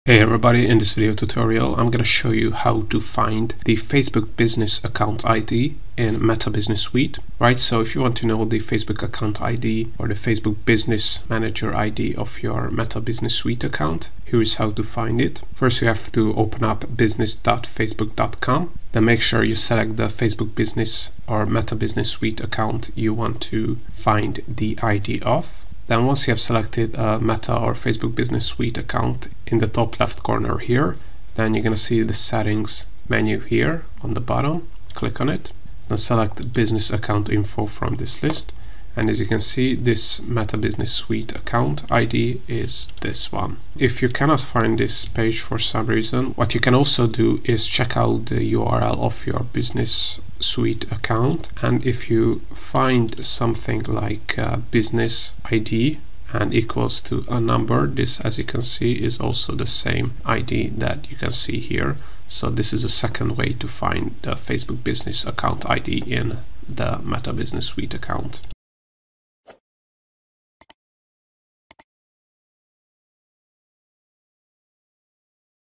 How To Find The Facebook Business Account ID In Meta Business Suite? [in 2024]
Learn how to find the Facebook Business account ID in Meta Business Suite.

In this video tutorial, we are going to show you How To Find The Facebook Business Manager ID in Meta Business Suite.

How To Find Facebook Business ID In Facebook Business Manager In 2023?
Steps to find the Facebook Business ID:

Open up Facebook Business Manager
Navigate to the Business Settings page
Click on Business Info
how to find Facebook Business ID

5. You can see the Facebook Business ID right at the top of the page:

Facebook Business Manager ID in Facebook Business Manager

How To Find The Facebook Business Manager ID In Your Browser Address Bar?
The second option to find the Facebook Business you have is basically the same, but actually, it is even easier. Here is how the complete process looks like which is the same in the first three steps:

Open up the Facebook Business Manager account you want to know the business ID of
Navigate to the Business Settings
Here you don’t have to click anywhere else, just have a look at the URL in your browser address bar.
Your Facebook business manager ID is the number after the URL part: “business-id=” as you can see in the picture below:
How to find Facebook Business Manager ID

You can check out these video tutorials if you prefer watching the step-by-step with some explanation:

What Is The Facebook Business ID?
Facebook Business Manager ID is a unique identifier connected to a Facebook Business Manager. A Facebook Business Manager is basically the container that helps you organize your Facebook Business Pages and Ad Accounts that are connected to a particular business.

So usually, a legal entity should create only one Facebook Business Manager, which can have many Facebook pages and Facebook Ad Accounts. However, you can get access to an unlimited number of Facebook Business Manager, so make sure when you find the Facebook Business Manager ID that you are in the right Facebook Business Manager account.

How To Find Facebook Business Manager?
You can find your Facebook Business Manager basically two main ways.

either open the URL business.facebook.com and log in into your Facebook account
or open your Facebook profile and click on ‘Business Manager’ from the left columnHow to find or access your Facebook Business Manager account
How To Create A Facebook Business ID?

Facebook Business Manager ID Vs Facebook Ad Account ID Vs Page ID Vs Pixel ID